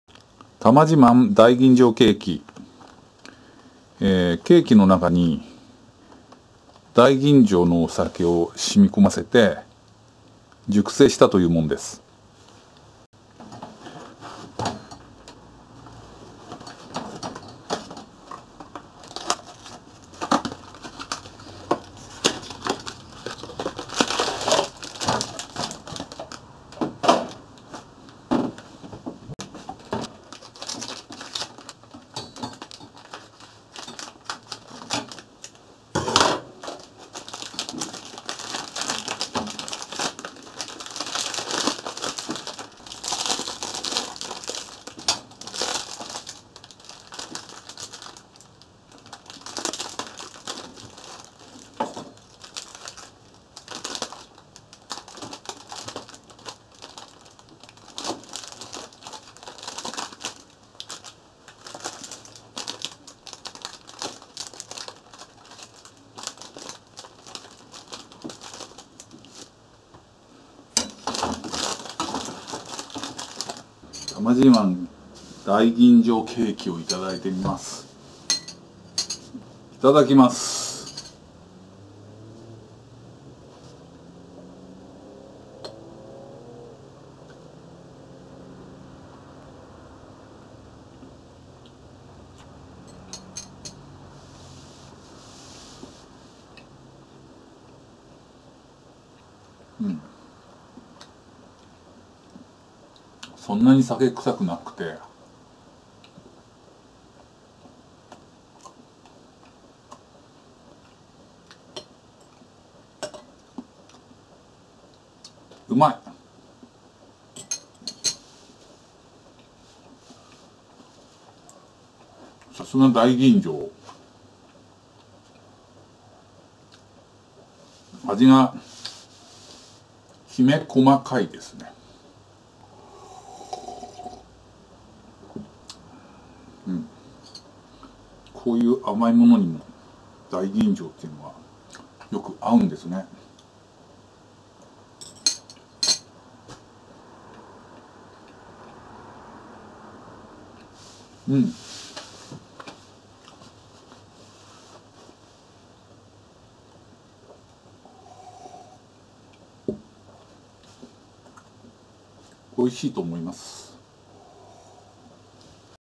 Daiginjo cake: 大吟醸ケーキ
多満自慢の大吟醸を浸み込ませて熟成したというケーキ。大吟醸酒の上品な香りが、ケーキによく合って美味しい。 This cake is aged after being soaked in daiginjo sake. Delicious!